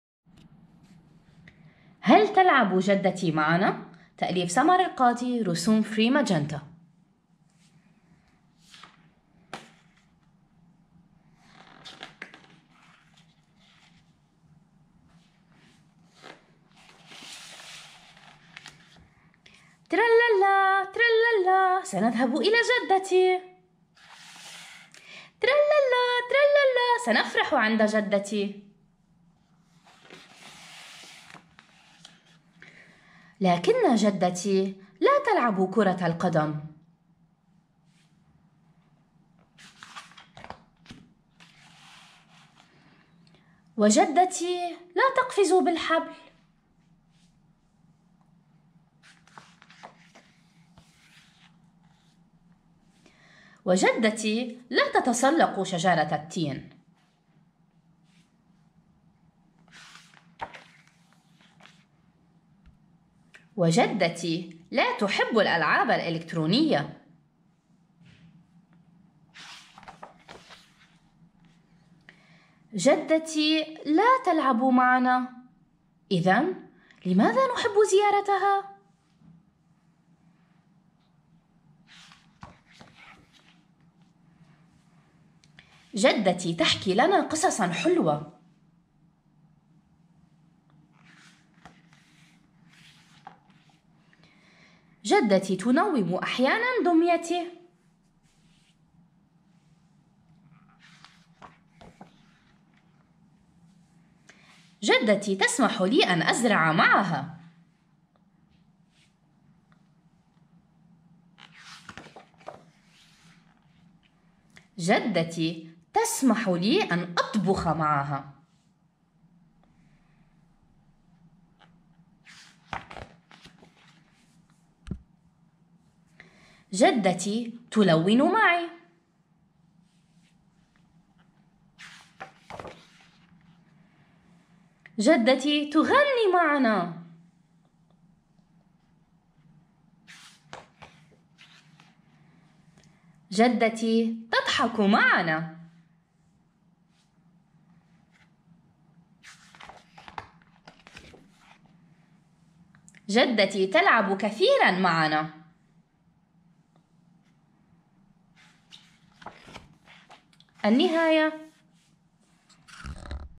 هل تلعب جدتي معنا؟
تأليف: سمر القاضي
رسم: فري ماجنتا
قراءة: جمال محفوظ
دار النشر: دار النديم
الموضوع: اللعب، العائلة